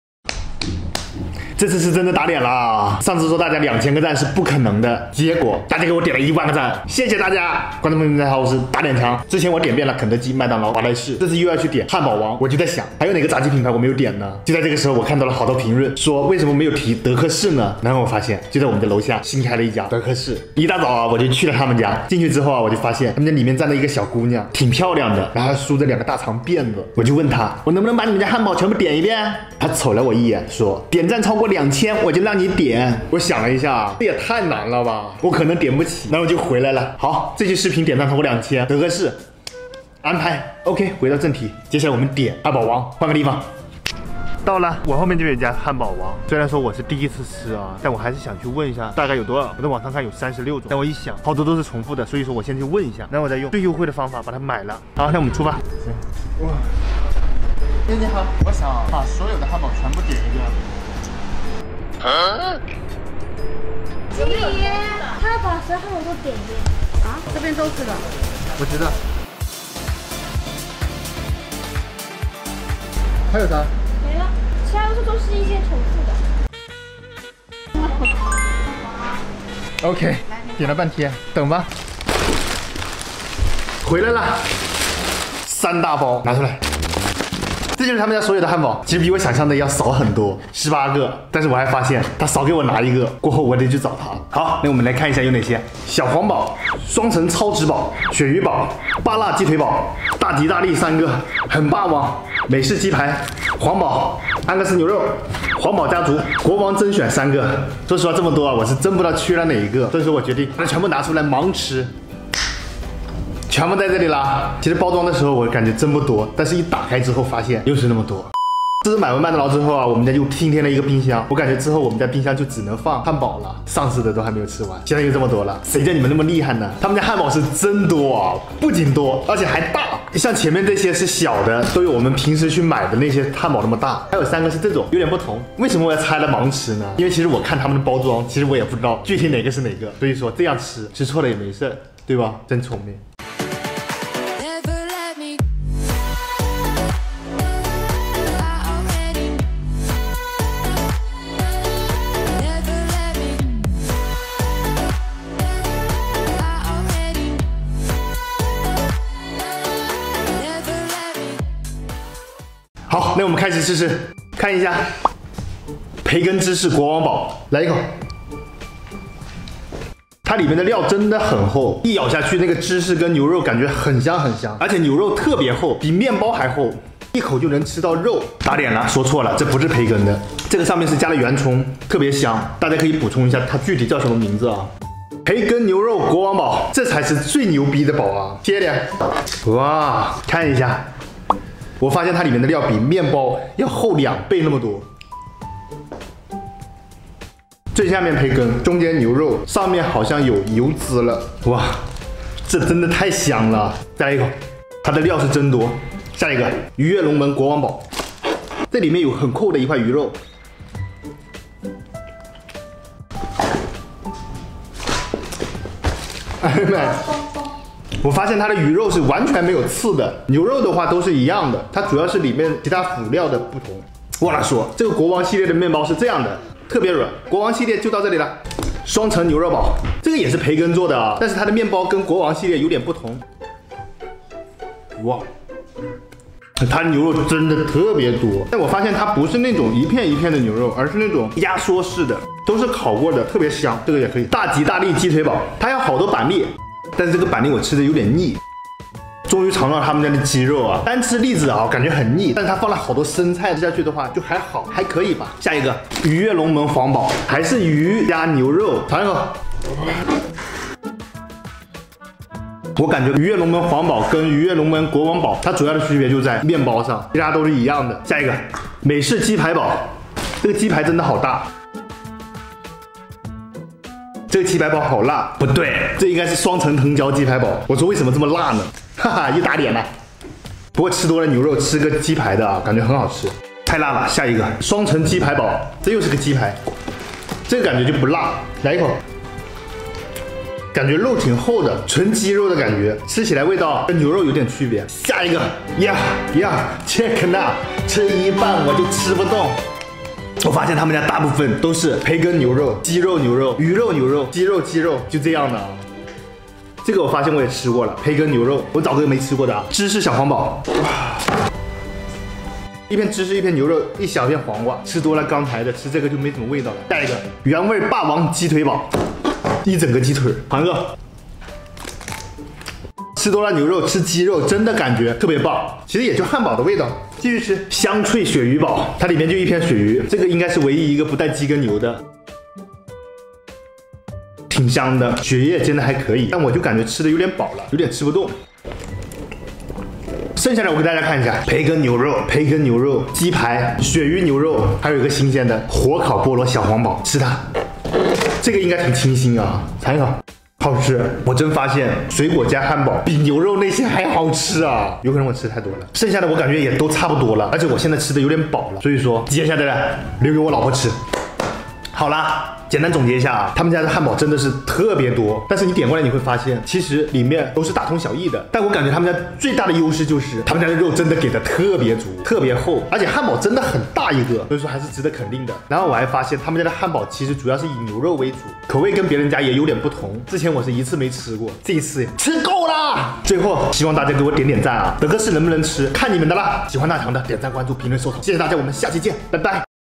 把汉堡王所有汉堡都买一遍要多少钱？经理亲自接待够牌面！
点光汉堡王~~ 给个关注，点个赞呗！